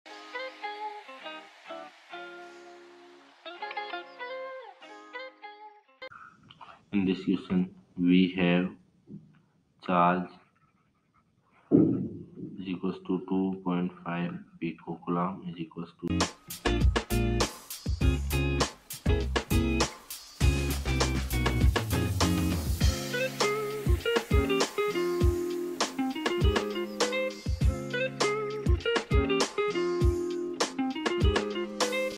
A 2.5 pC charge is initially moving at a speed of 200 m/s at an angle of 43Â° N of E in a uniform m…
A 2.5 pC charge is initially moving at a speed of 200 m/s at an angle of 43Â° N of E in a uniform magnetic field directed directly up (N). a) Sketch the situation. b) Find the magnitude of the force exerted on the charge. c) Find the direction of the force.